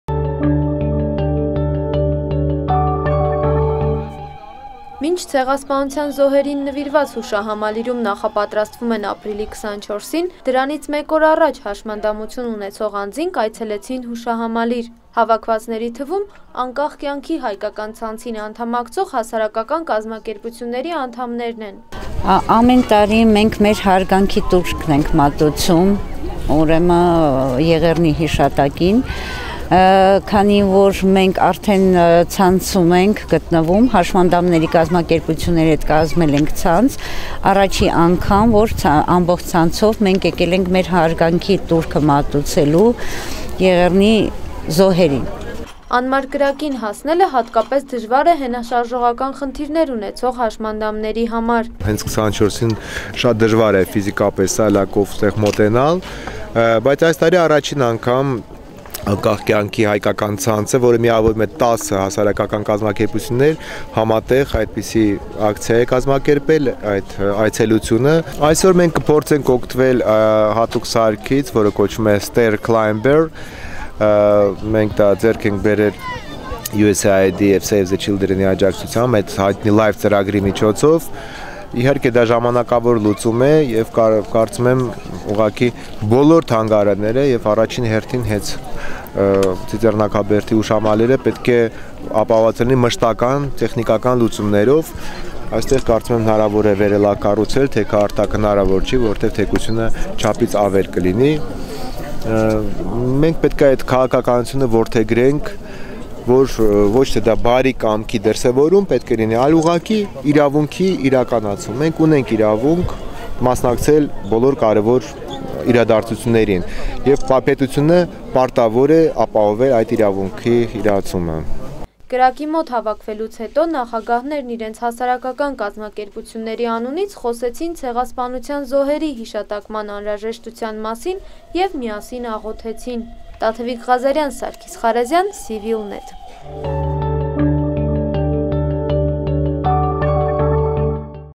Մենք էլ իրավունք ունենք
Հաշմանդամություն ունեցող անձանց ապրիլի 24-ին դժվար է բոլորի հետ երթով բարձրանալ Ծիծեռնակաբերդ: Այդ պատճառով «Անկախ կյանքի հայկական ցանցին» անդամակցող հասարակական կազմակերպություններն ապրիլի 23-ին այցելեցին հուշահամալիր: